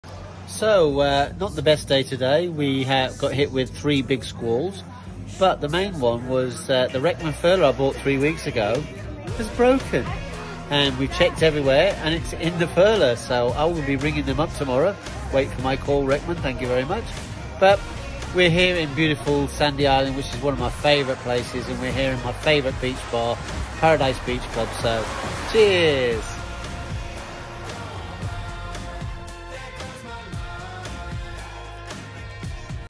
New Reckmann Furler Broken!
Our new Reckmann Furler has broke! Thank goodness I’m in paradise. Otherwise, I might just lose my mind!